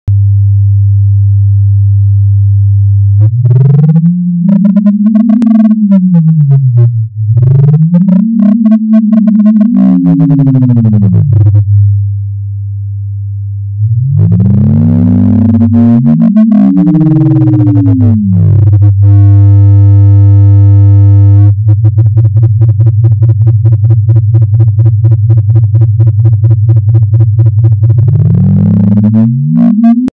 Webcam instrument (Winstrument?) - Video 2
Updated the application a bit. Now the music sounds better, the code is cleaner and it can do a bit more.